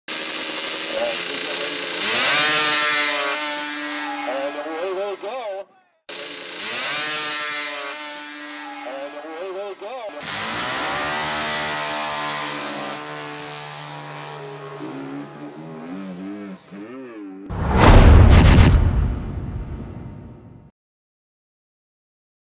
Snowmobile Race | Off To A Bad Start
This snowmobile rider got a jump start in this race when he crashed into the starting light. He was clearly eager to get going, but didn't make it too far.